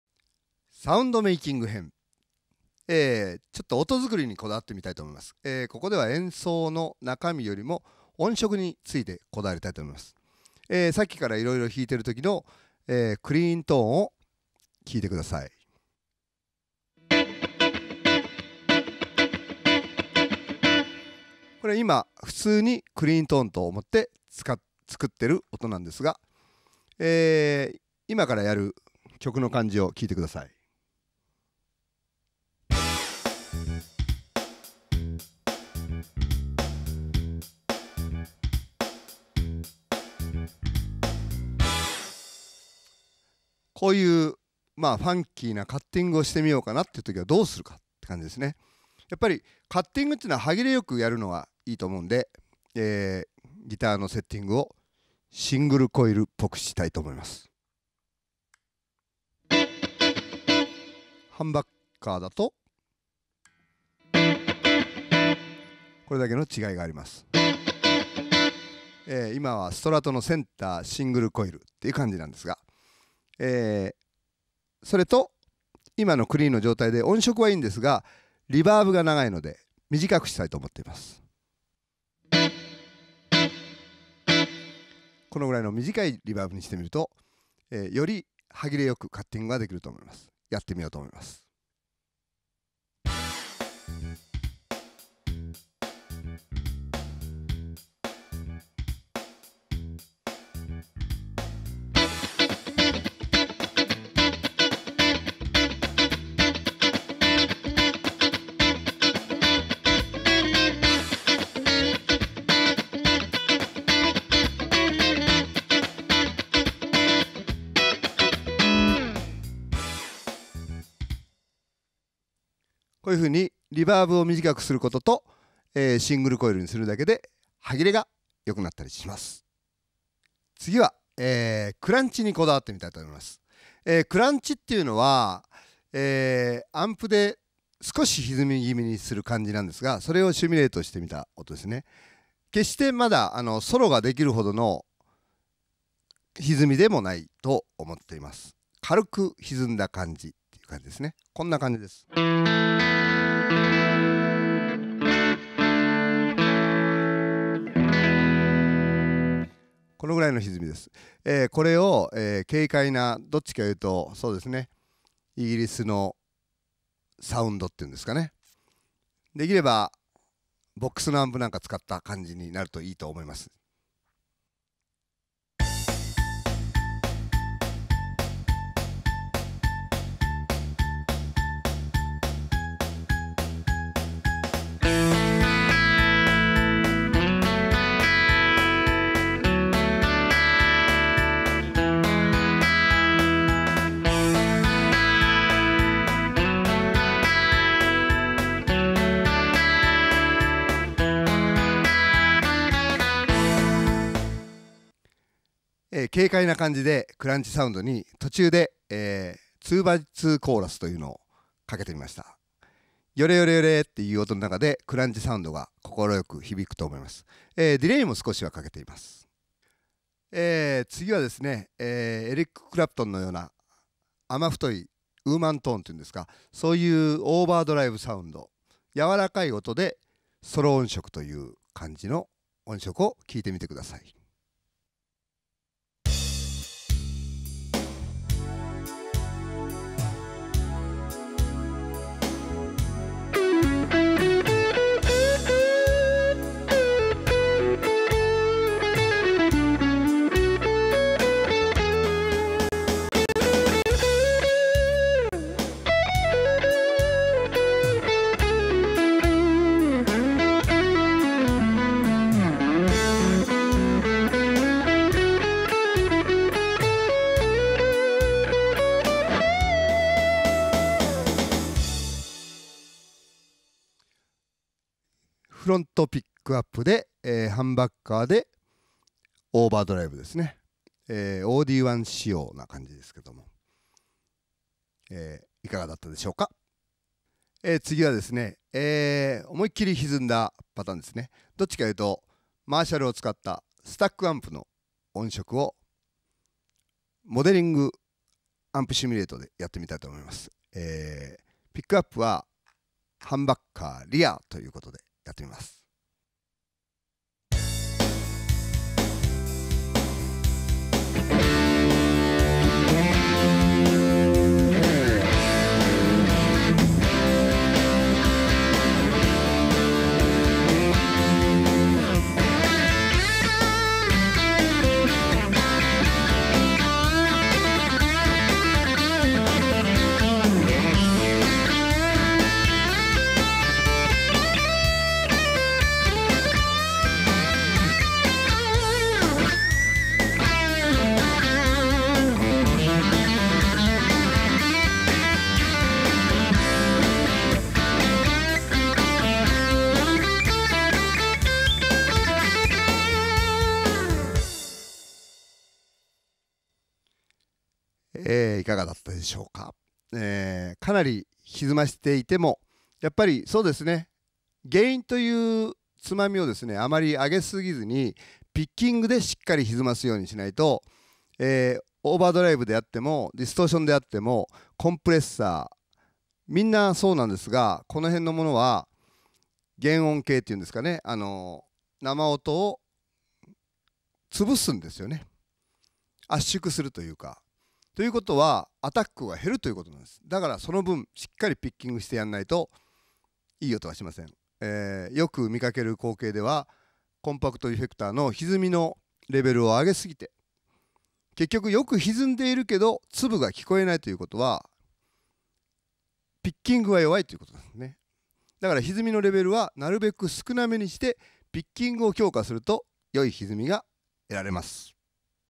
岩見和彦（NANIWA EXP） 直伝！ジャンルごとの音作り
Amazonビデオで好評配信中！

こちらの映像は教則DVD『岩見和彦 直伝 クロス・オーバー・ギター・プレイ』に収録されております。

浪花が生んだ最強のライヴ・バンド:NANIWA EXPのギタリストとして活躍する他、
ジャズからJ-POPまであらゆるジャンルを弾きこなす岩見和彦。
その真の意味での“クロスオーバー”なギター・プレイを
本人自ら解説する教則DVDが登場！

スムーズな運指のためのエクササイズの他、
テンション・コード、様々なジャンルに 対応するコツを
ギター講師としても定評のある岩見和彦がわかりやすく解説します。
このDVDを見ればどんな音楽も自由自在。
あなたのプレイの幅が驚くほど広がります。